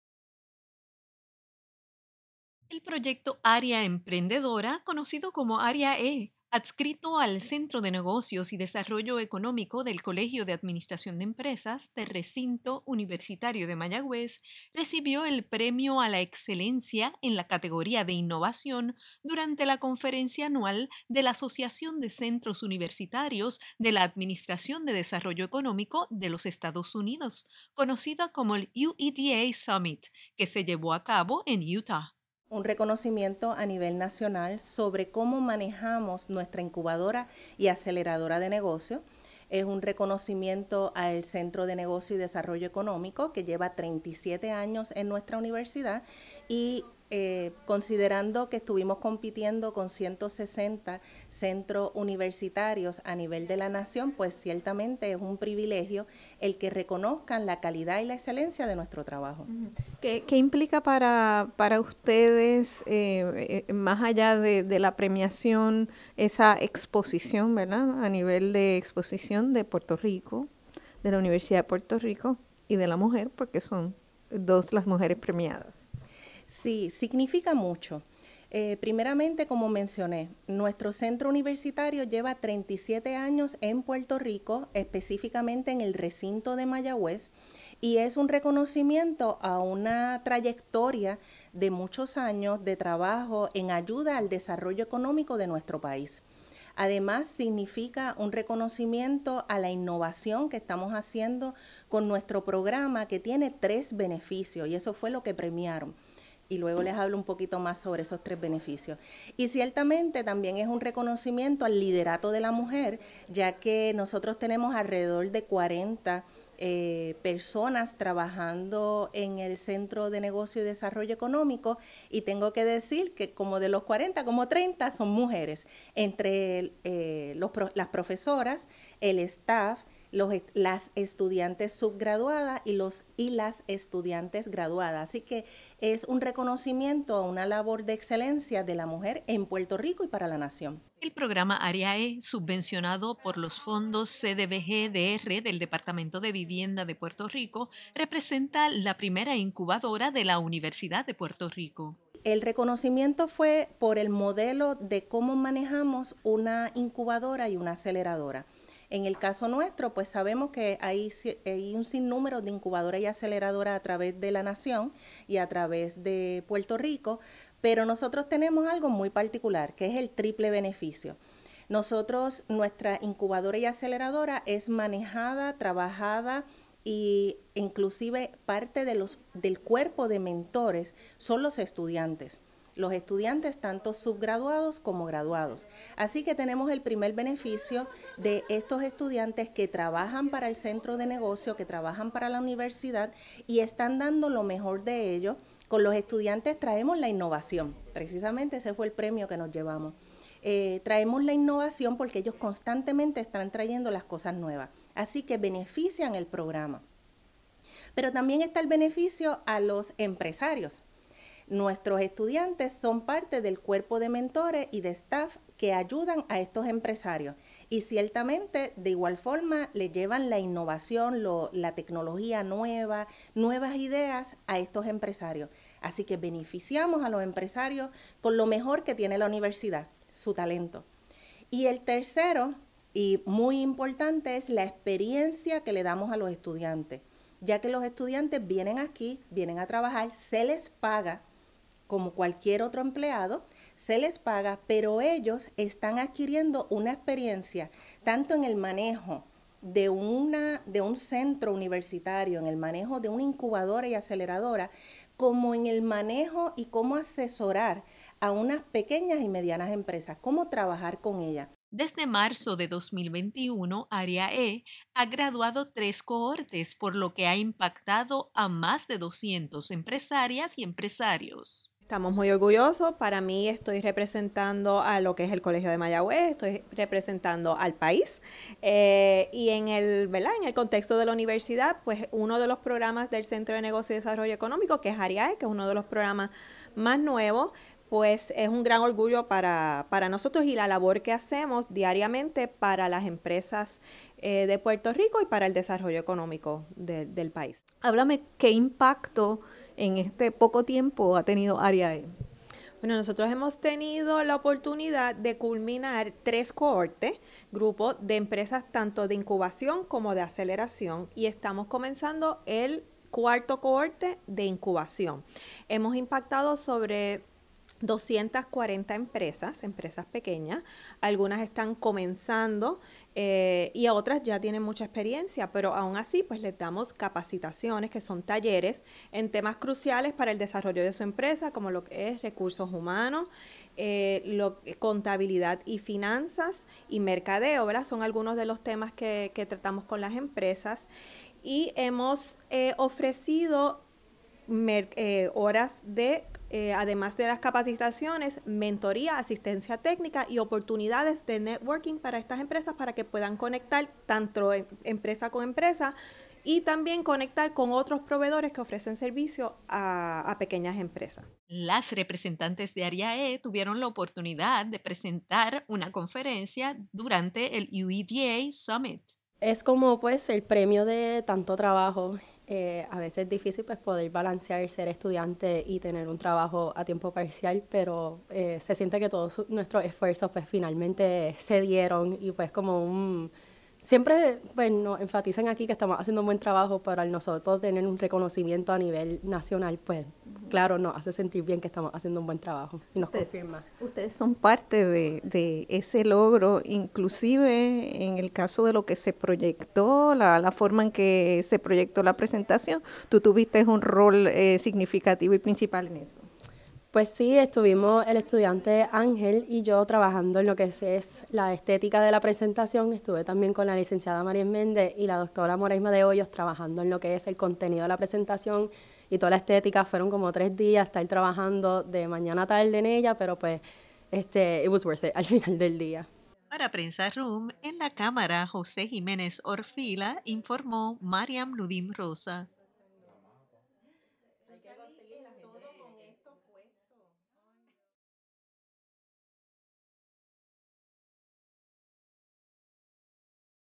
Área-E recibe Premio a la Excelencia en Innovación en el UEDA Summit 2023
El proyecto Área Emprendedora (ÁREA-E), adscrito  al Centro de Negocios y Desarrollo Económico del Colegio de Administración de Empresas (ADEM) del Recinto Universitario de Mayagüez (RUM) recibió el Premio a la Excelencia en la categoría de Innovación durante la conferencia anual de la  Asociación de Centros Universitarios de la Administración de Desarrollo Económico de los Estados Unidos, conocida como el UEDA Summit 2023, que se llevó a cabo en Utah.